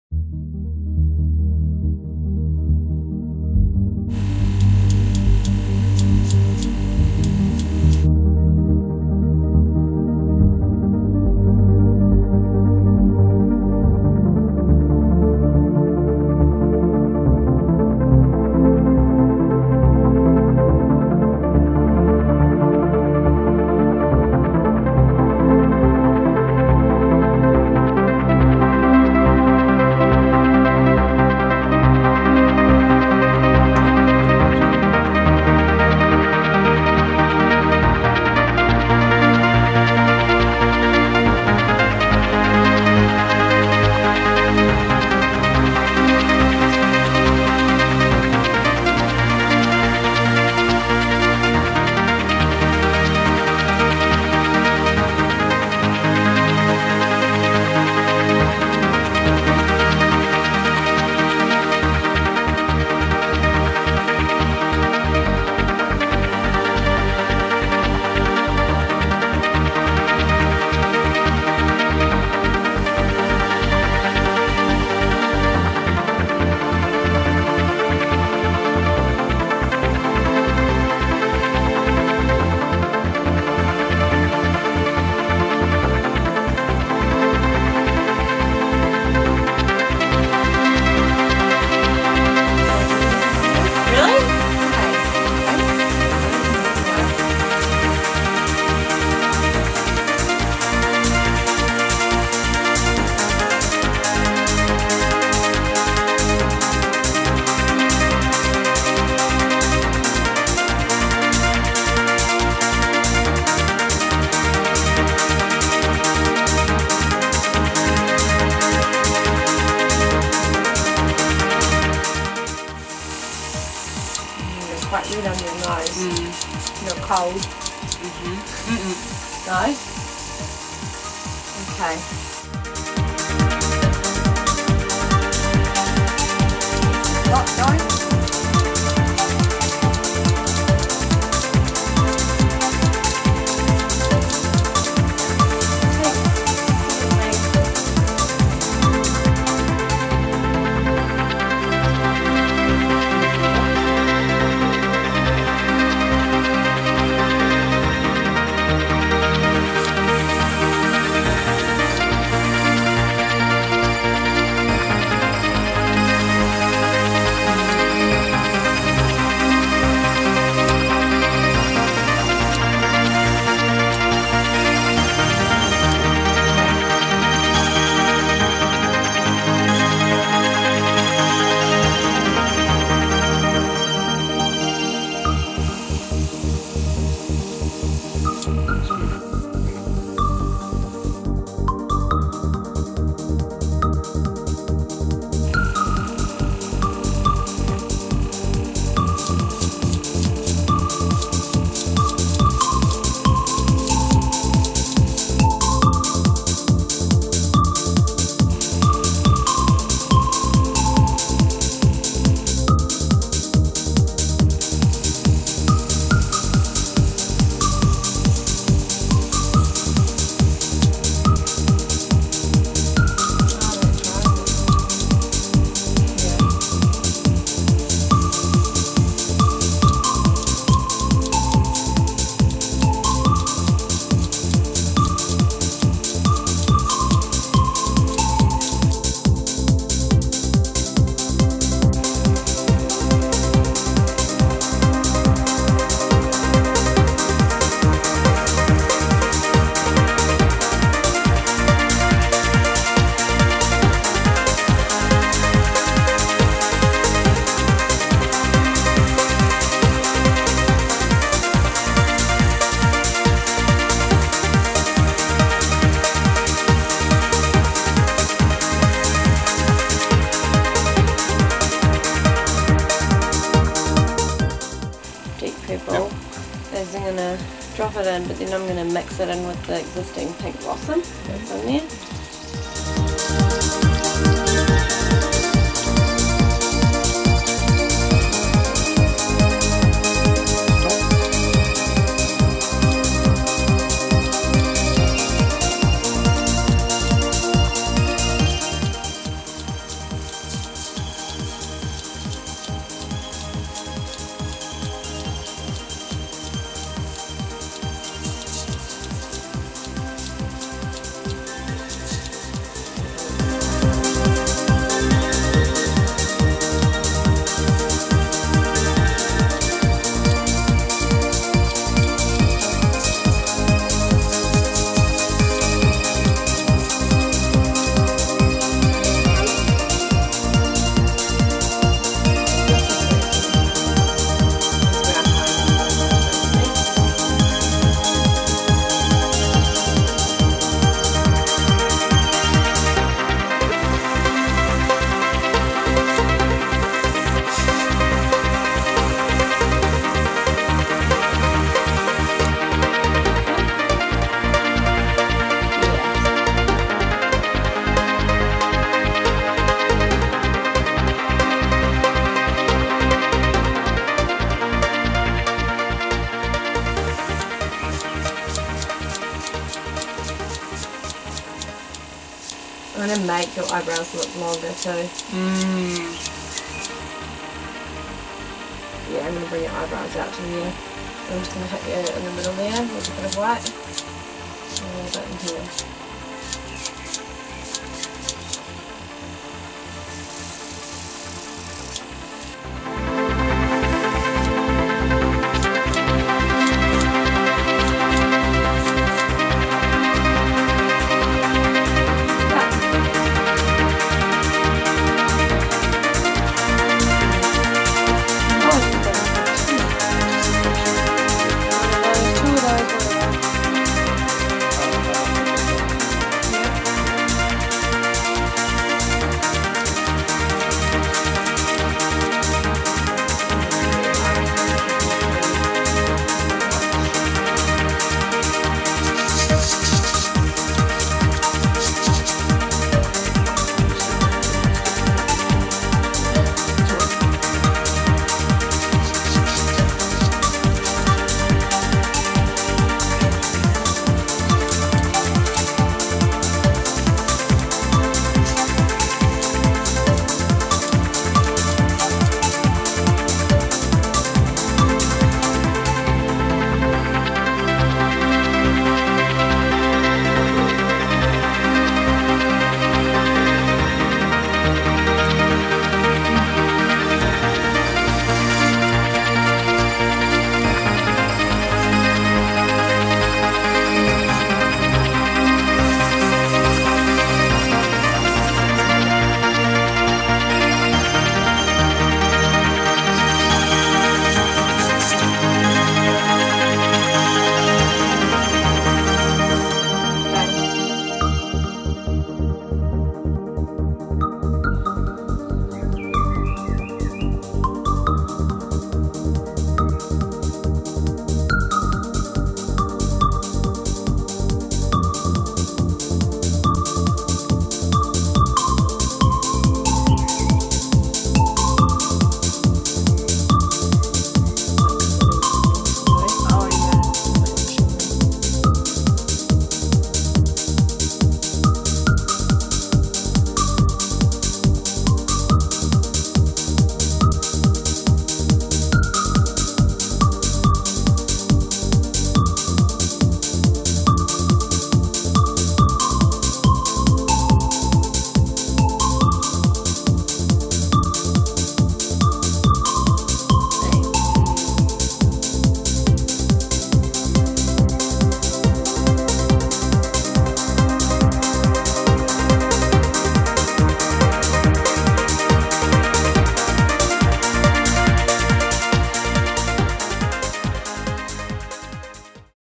Dinair Airbrush Make Up - Shimmer with Pinks & Purples
Watch Airbrush make up from start to finish - a complete look